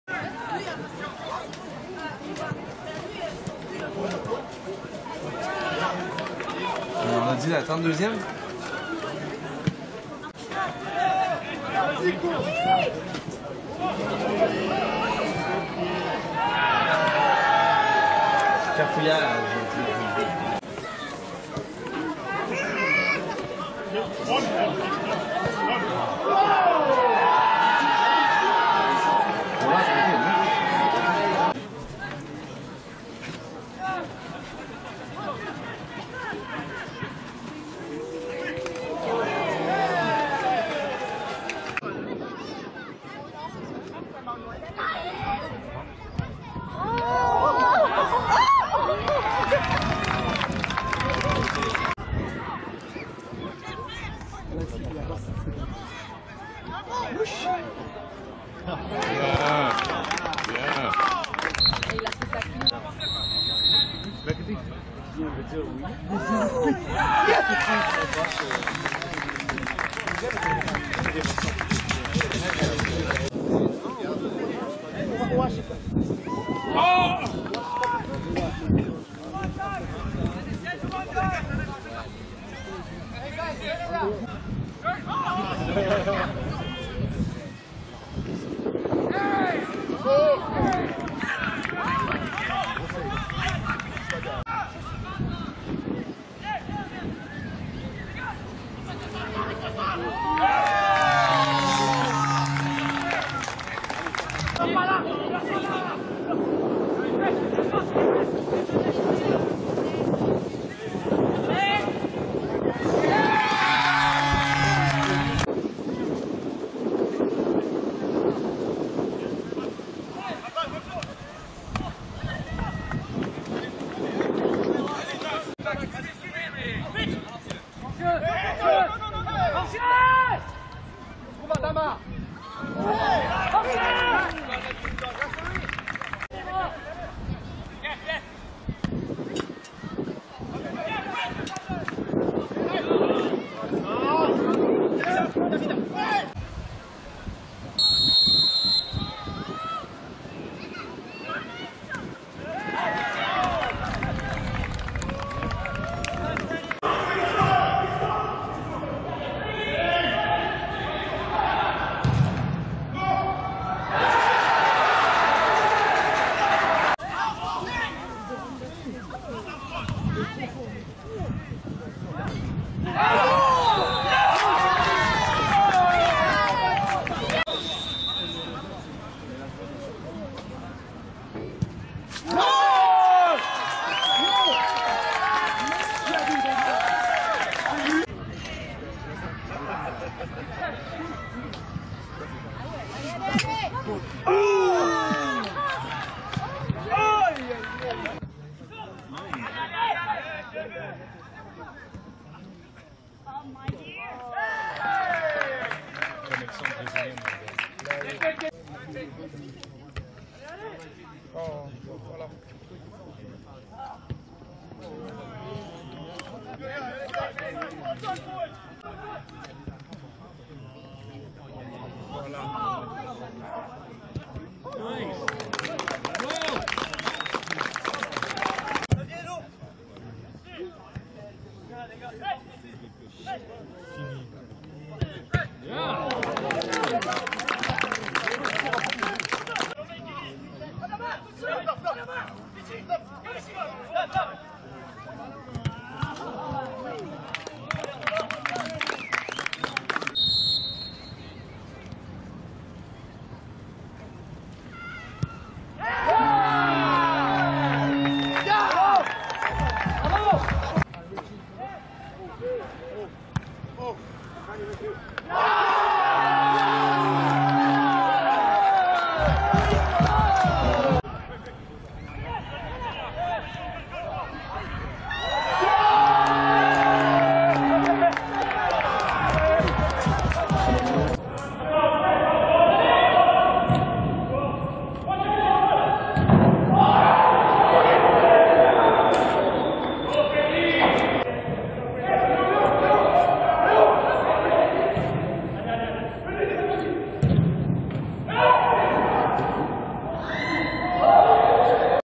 Frederico Moojen 2017 PLSQ Highlights
11 goals in 13 games during the 2017 PLSQ season